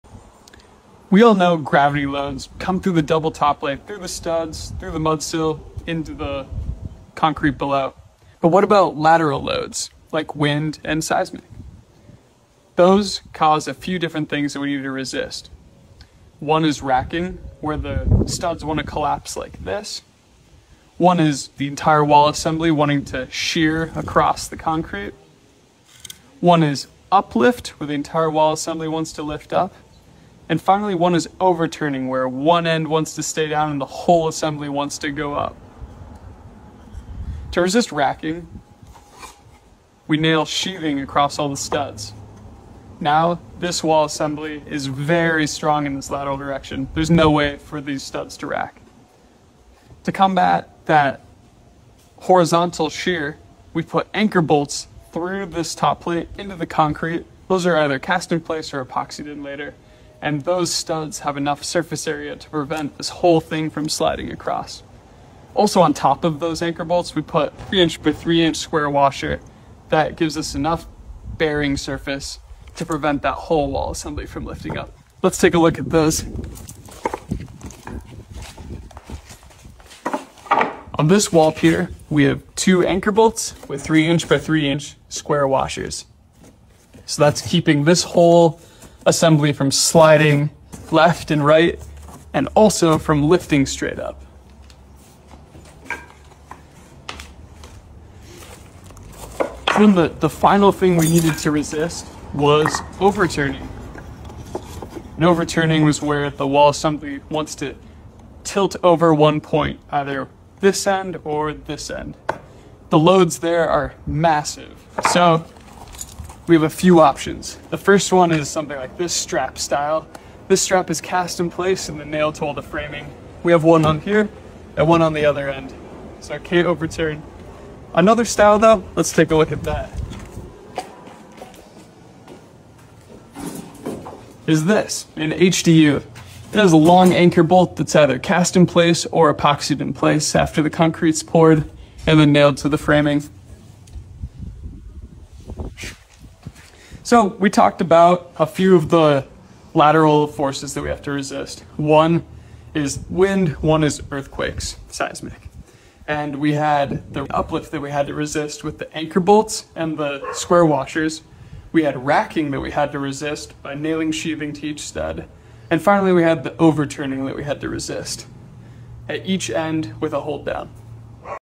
Most Homeowners Don’t Know This But It’s What Keeps Your House From Collapsing
How Wood Shear Walls Resist Large Wind & Earthquake Loads

🔹 Racking – Lateral forces try to collapse walls into a parallelogram shape. Shear panels (APA-rated plywood or OSB) nailed in a tight pattern stiffen the wall pier.

👉 Inspection tip: Check that shear panels are blocked at seams and nailing matches the engineer’s schedule.

🔹 Overturning – As wall piers resist lateral loads, one end of the wall pushes down while the other end lifts off. Holdowns at each end of the wall pier prevent each end from lifting off.

👉 Inspection tip: Verify holdowns are the correct size and in the right location per the engineering drawings and schedule.

🔹 Horizontal shear at base of wall – Forces from roof and floor diaphragms must transfer through the wall. In new construction, steel connectors like Simpson A34 and A35 clips connect blocking and rim joists to top plates, and nails connect sheathing to top plates, studs, and bottom plates. Anchor bolts prevent the bottom plate from sliding across the top of the stem wall.

🔹 Uplift – Wind or seismic motion can lift the roof and floor diaphragms off the walls. Metal straps and clips tie the assemblies together. 3” square washers on anchor bolts extending through the bottom plate prevent the entire wall assembly from lifting off the concrete stem walls.

👉 Inspection tip: Confirm all straps and clips are installed in the correct locations with the correct fasteners per the engineer’s drawings and schedule. Confirm 3” square washers are installed on every anchor bolt.